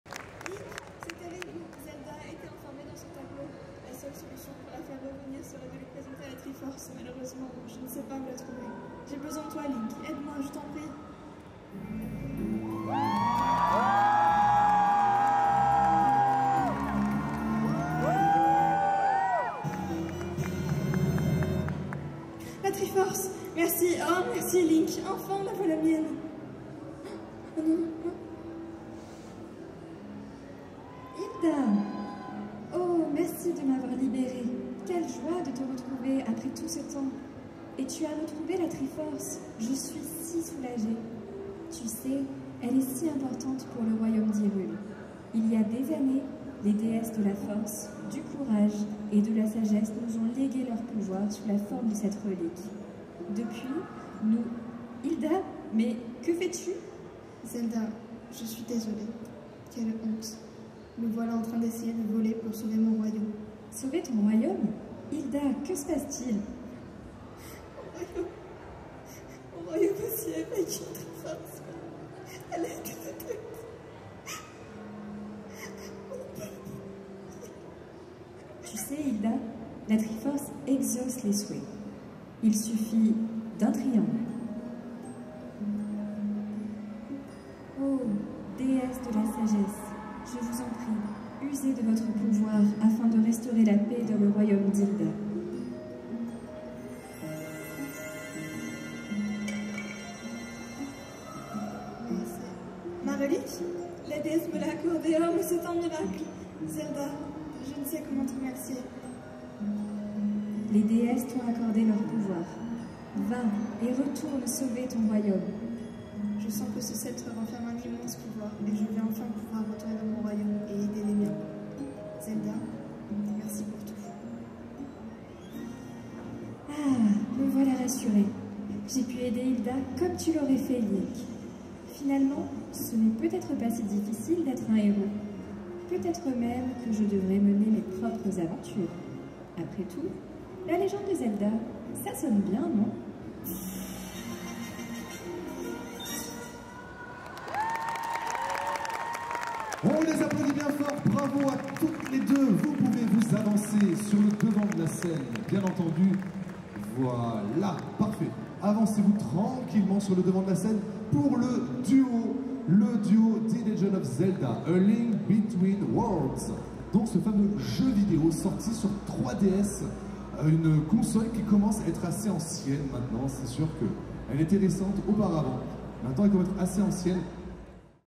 TGS 2023 - French ICL Selections - Hilda & Zelda (The Legend of Zelda)
TGS Toulouse 2023, Saturday, French ICL Selections

Cosplay : Hilda & Zelda (The Legend of Zelda)
Cosplayer :